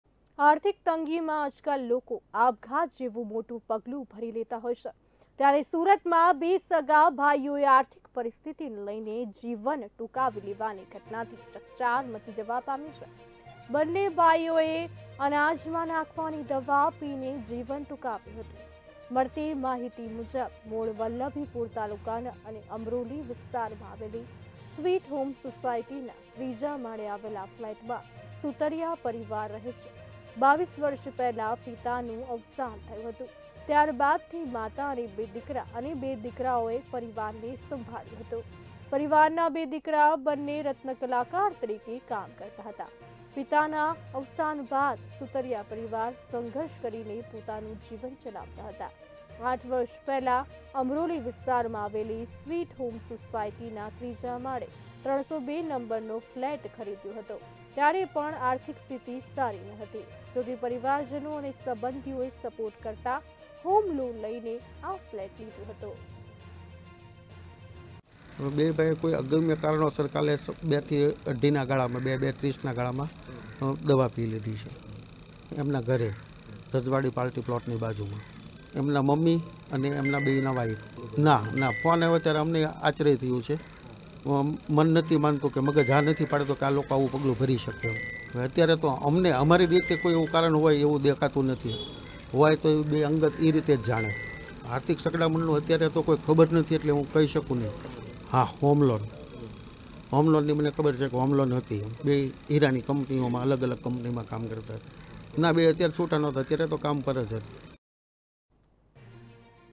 સુરતમાં બે સગા ભાઈઓએ આપઘાત કરી લીધો.Two brothers commit suicide in Surat
He committed suicide due to poor financial condition.
Brothers end life by consuming poison
The matter was informed to police.
Police has been investigating further into the case.